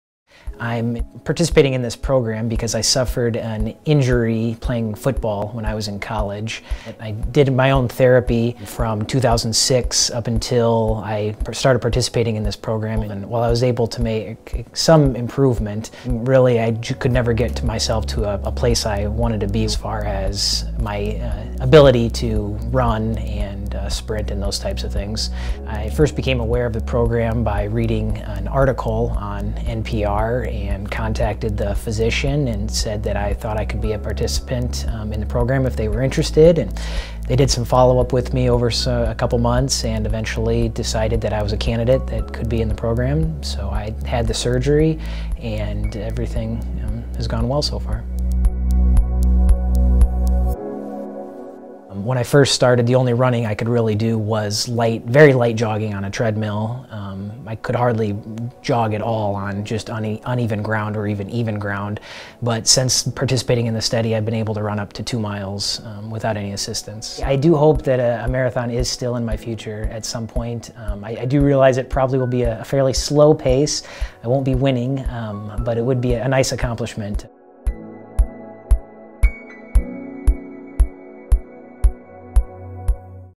Regenerative Medicine Improves Function in Severe Muscle Injuries | UPMC
Maury Noonan of Mason City, IA, discusses his experience participating in the Muscle Tendon Tissue Unit Repair and Reinforcement Reconstructive Surgery Research Study.
Research showed significant improvement in strength and range of motion, as well as evidence for skeletal muscle regeneration.